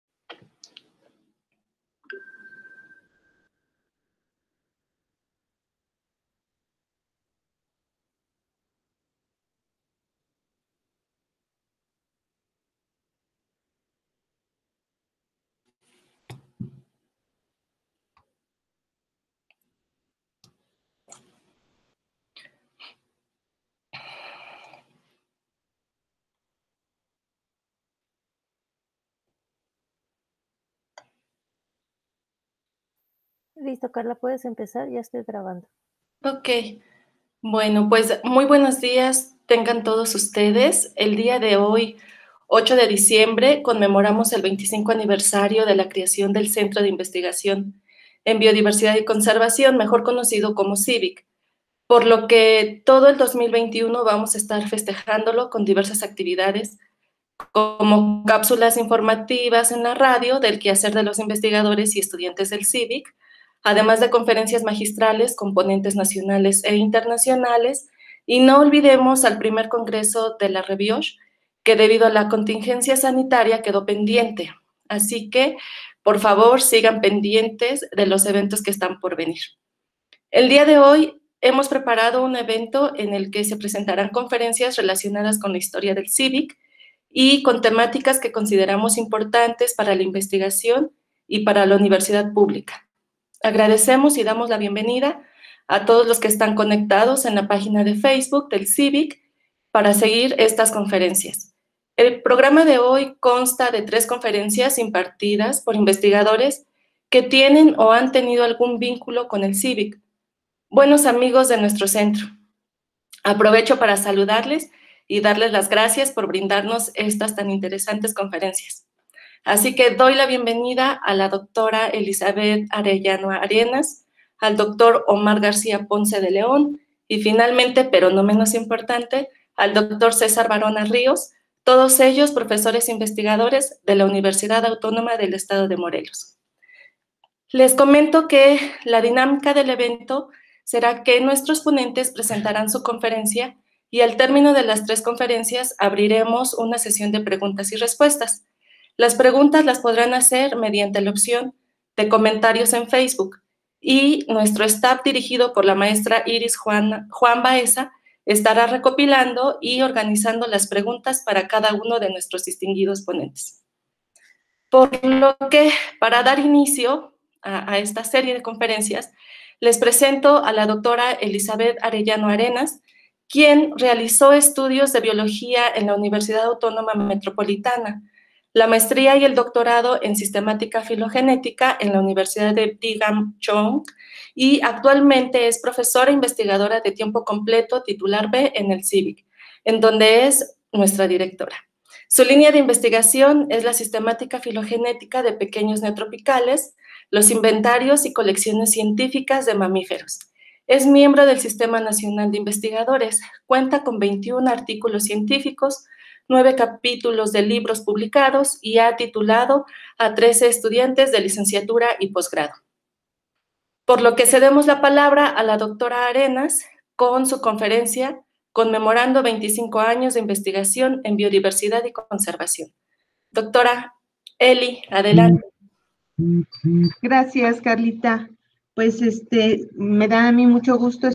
Introducción. Evento Aniversario CIByC 2020 12 08
Introducción al evento del 25 Aniversario del Centro de Investigación en Biodiversidad y Conservación (CIByC) Por Dra. Karla Aguilar Dorantes